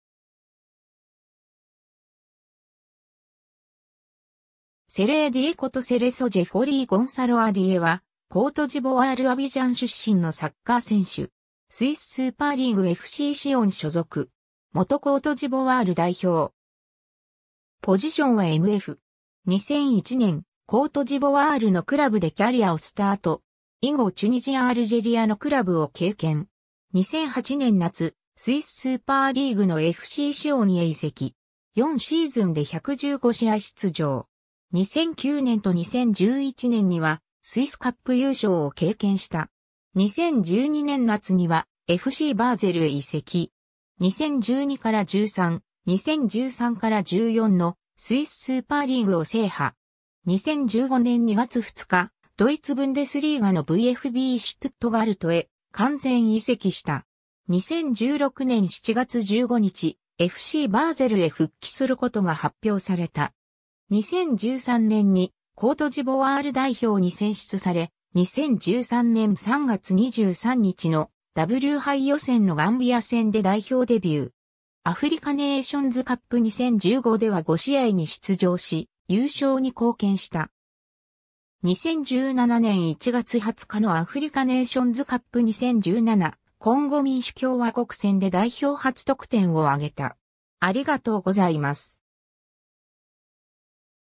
セレイ・ディエ
ヌーシャテル・ザマックスの選手
セレイ・ディエ（Serey Dié）ことセレソ・ジェフォリー・ゴンサロア・ディエ（Sereso Geoffroy Gonzaroua Die、1984年11月7日 - ）は、コートジボワール・アビジャン出身のサッカー選手。
スイス・スーパーリーグ・FCシオン所属。
元コートジボワール代表。
 2001年、コートジボワールのクラブでキャリアをスタート。
以後チュニジア・アルジェリアのクラブを経験。
 2008年夏、スイス・スーパーリーグのFCシオンへ移籍。
4シーズンで115試合出場。
2009年と2011年にはスイス・カップ優勝を経験した。
　 2012年夏にはFCバーゼルへ移籍。
2012-13, 2013-14のスイス・スーパーリーグを制覇。
 2015年2月2日、ドイツ・ブンデスリーガのVfBシュトゥットガルトへ完全移籍した。
 2016年7月15日、FCバーゼルへ復帰することが発表された。
 2013年にコートジボワール代表に選出され、2013年3月23日のW杯予選のガンビア戦で代表デビュー。
アフリカネイションズカップ2015では5試合に出場し優勝に貢献した。
 2017年1月20日のアフリカネイションズカップ2017・コンゴ民主共和国戦で代表初得点を挙げた。